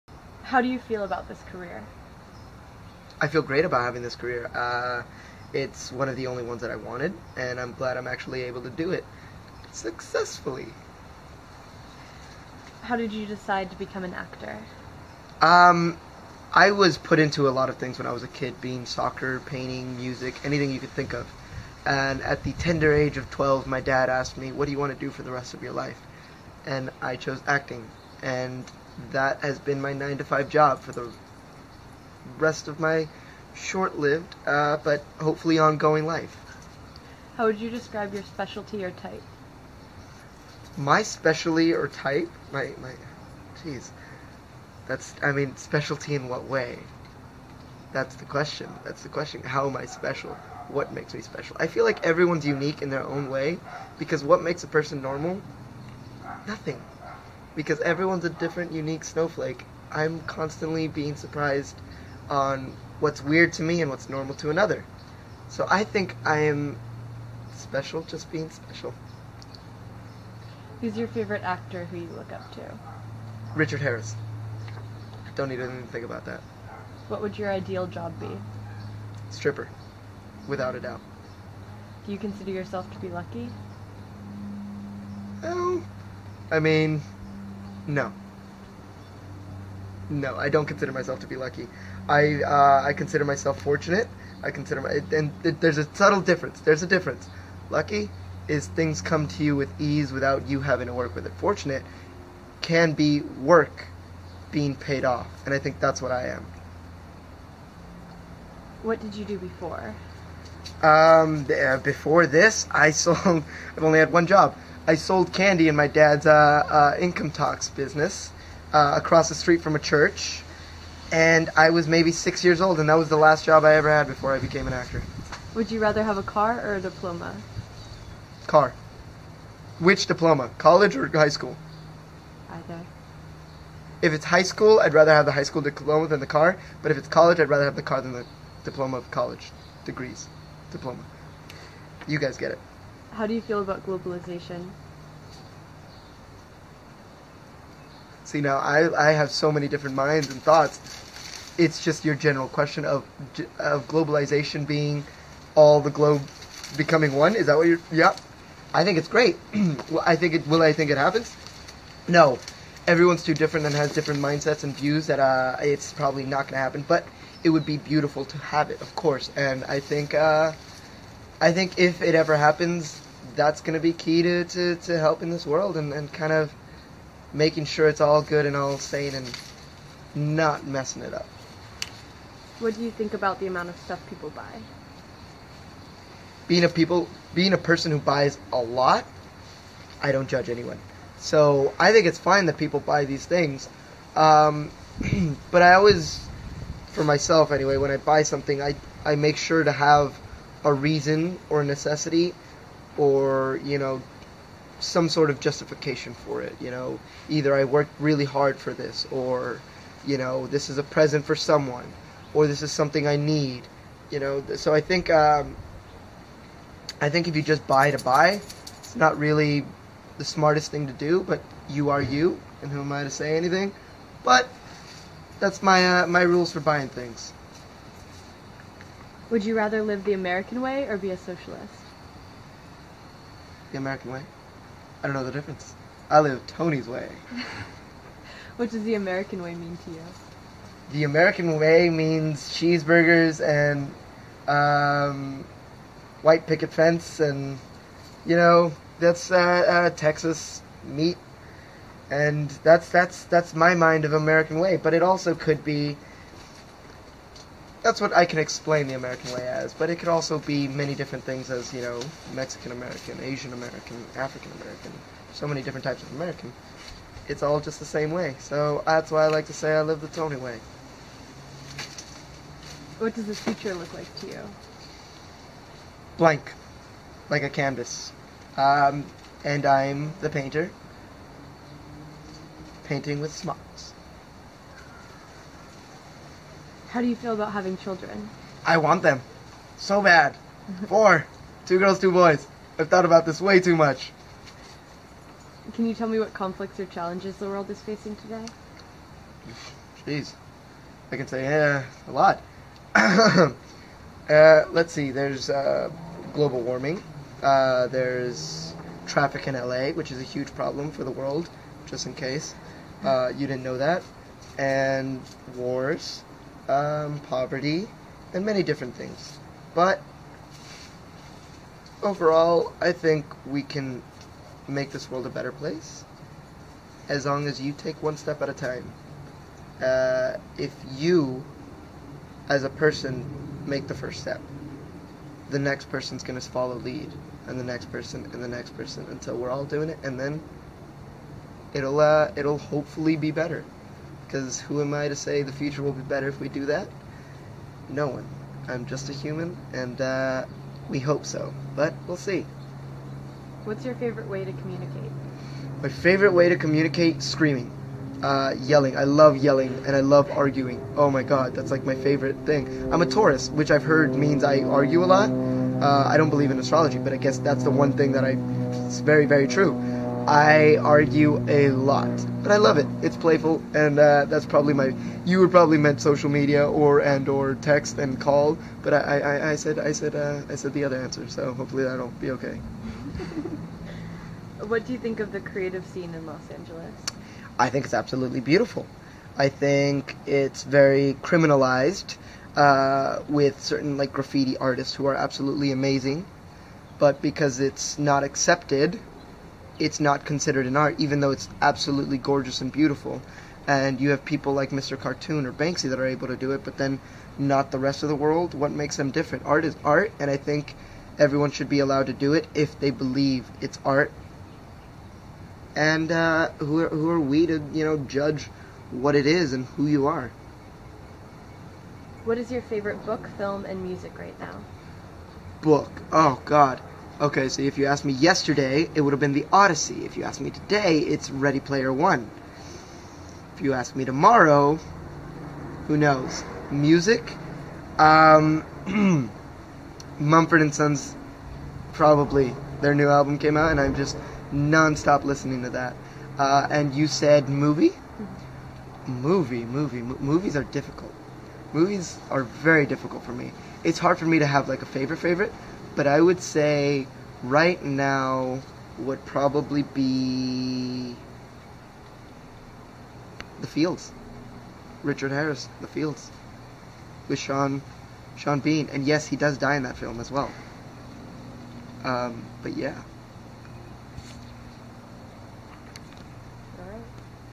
Tony Revolori Go-See Interview
Best known for playing the role of Zero Moustafa in The Grand Budapest Hotel, Tony Revolori will appear in the upcoming Sundance hit Dope, as well as Table 19, When We Were Pirates, Temps, and the 2016 sci-fi film 5th Wave.

Images and Video by Jan-Willem Dikkers